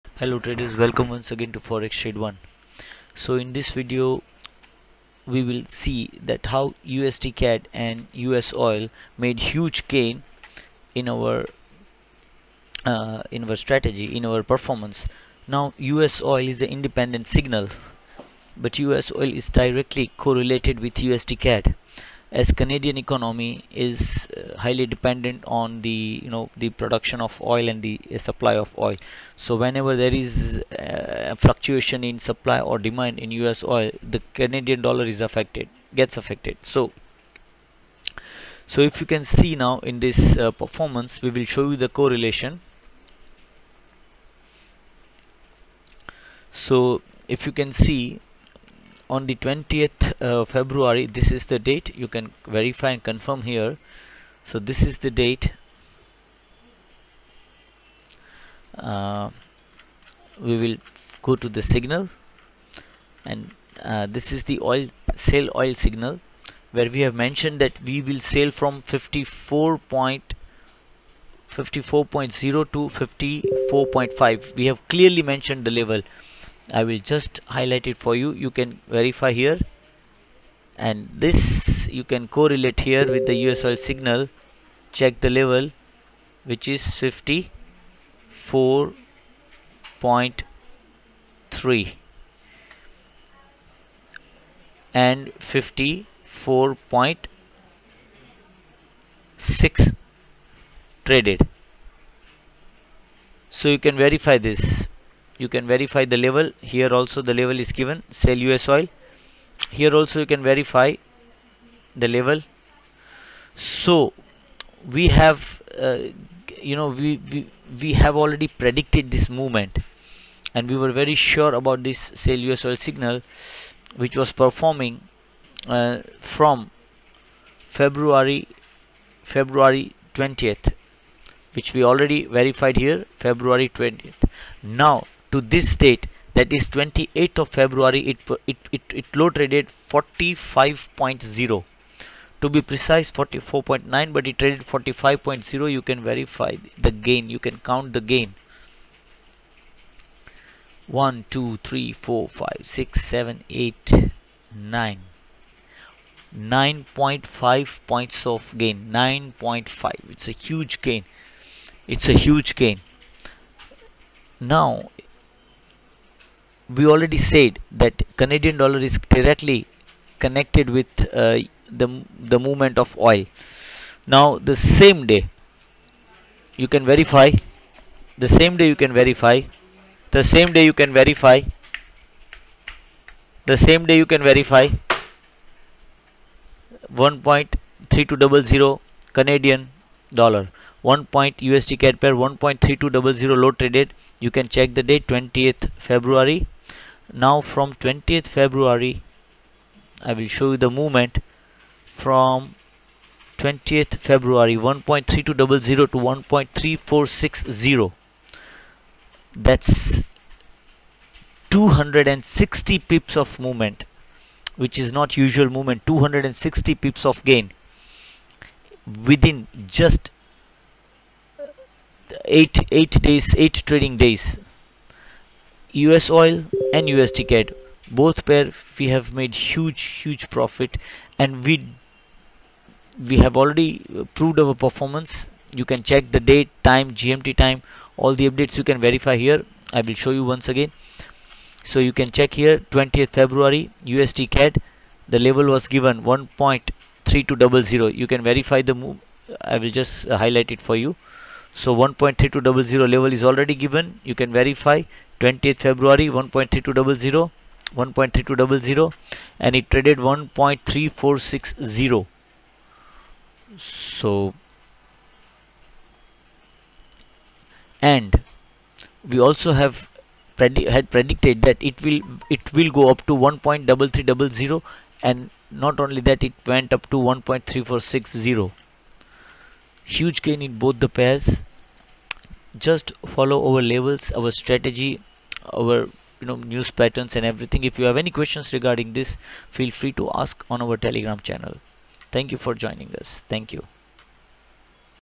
USDCAD & USOIL performance 20Feb to 28Feb
Check our 20th Feb Both BUY USDCAD SIGNAL and SELL USOIL signal

USOIL is our Independent signal but USOIL and USDCAD both are directly related to each other.

CANADA is one of the highest producer and supplier of crude, and thereby OIL prices affect loonie instantly

WE predicted the USOIL movement and bought USDCAD with high lot sizes.

Verify our Claim herewith the given link.
SELL USOIL
54.0 to 45.0 = 9.0 points of gain

1.3200 to 1.3460 = 260 pips of gain

Connect your Account with our CopyTrade service and get the same advantage to your account.